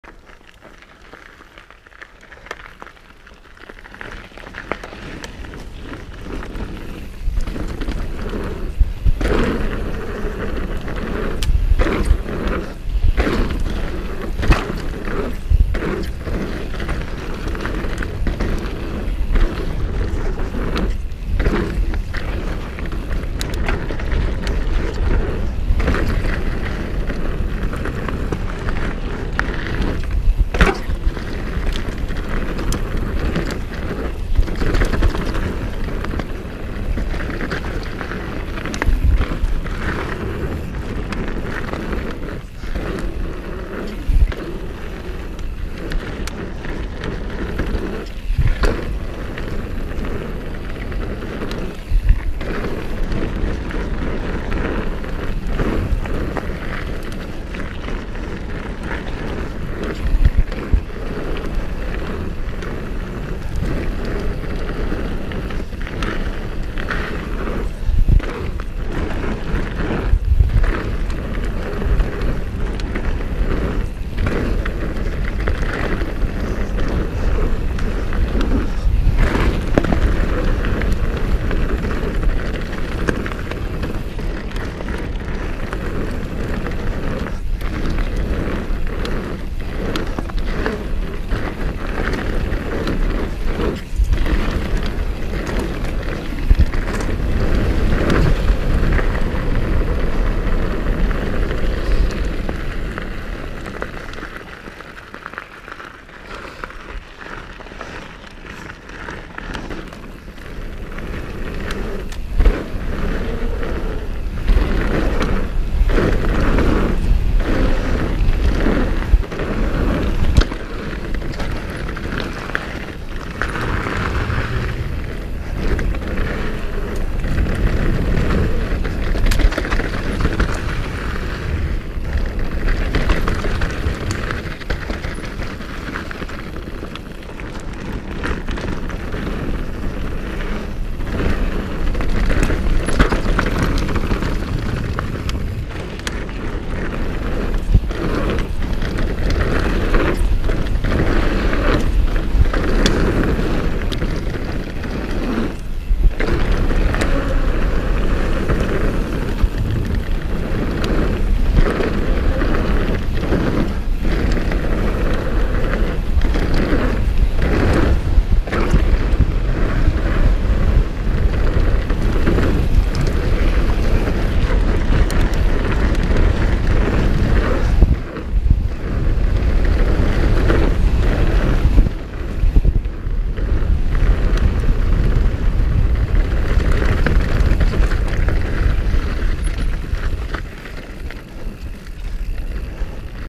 Forest of Dean MTB - Countdown into Launchpad
Downhill bike is overkill for these trails but I needed to get back to my van and I haven't ridden either in a while.

Both are fun and suitable for pretty much everyone.